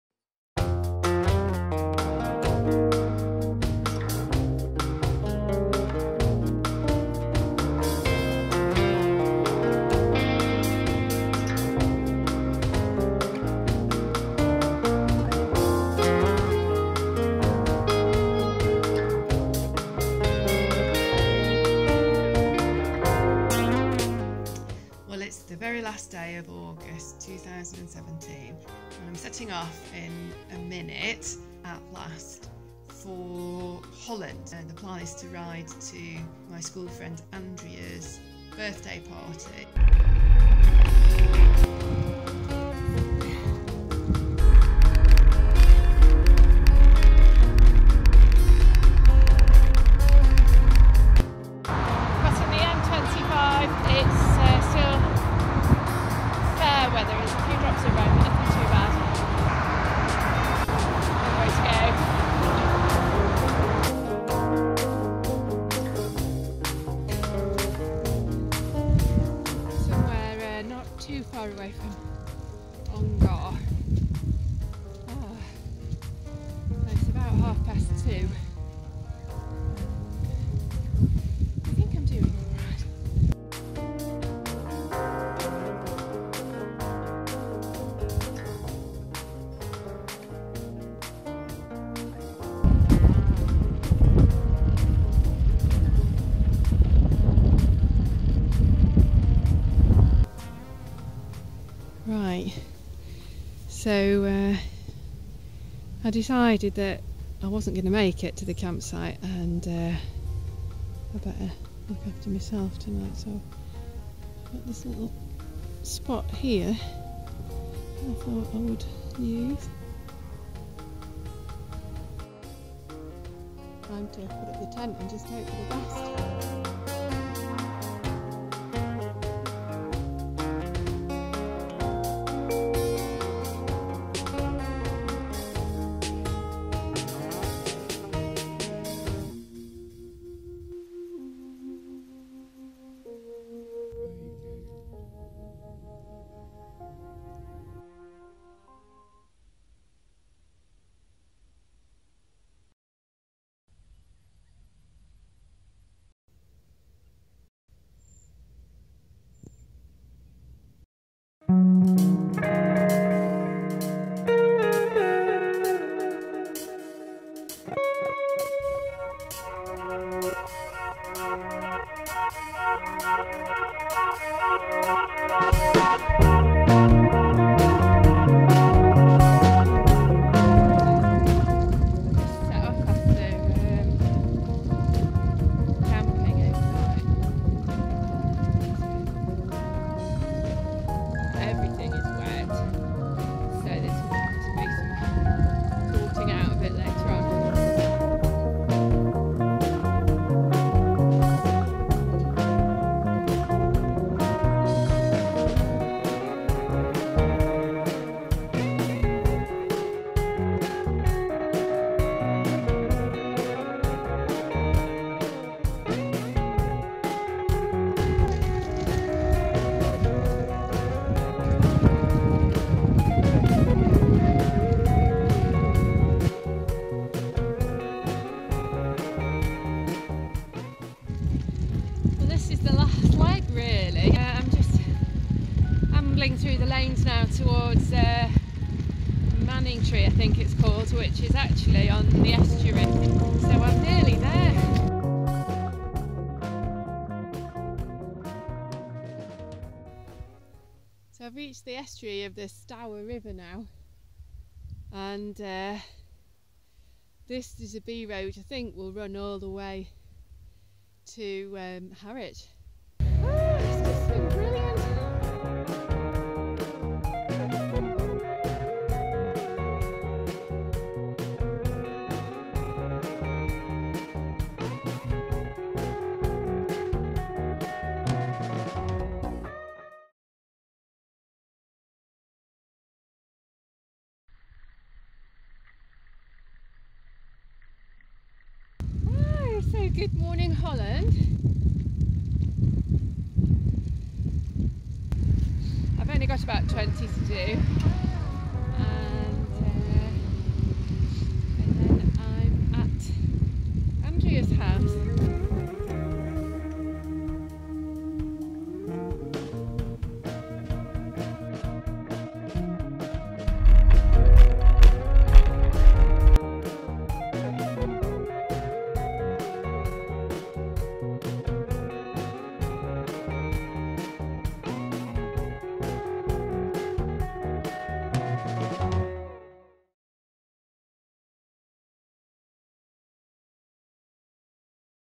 By Brompton London to Holland - with my tent - a 2.5 day cycle ride to my friend's birthday party
This was a camping trip on a Brompton folding bike, a leisurely, late-summer ride from London to the Hague. First I rode an easy couple of days through Essex, which was surprisingly gorgeous. I didn't quite make it to the campsite I was aiming for, so found a spot to camp wild. I woke up to a fantastic sunrise, and then pedalled on down the lanes to the coast. At Harwich, I folded up the Brompton, and went on board the night ferry to Hook of Holland, in the Netherlands. The next morning, I could just unfold the bike, and bowl along the sunny beaches on the Dutch coast. I arrived at my schoolfriend Andrea's home in the Hague, tucked my bike and tent in a corner, changed into my posh togs (a rolled-up Little Black Dress fits quite nicely into a pannier), and joined the party 🎉✨   Hope you enjoy the film!